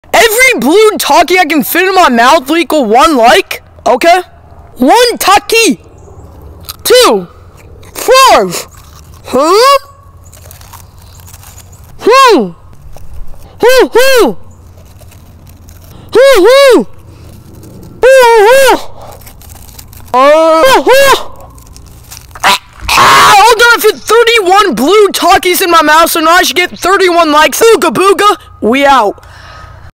I ATE REALLY SPICY CHIPS FOR THIS🥵
I FIT 31 BLUE HEAT TAKIS IN MY MOUTH IT WAS SUPER SPICY! HOW MANY TAKIS COULD YOU FIT?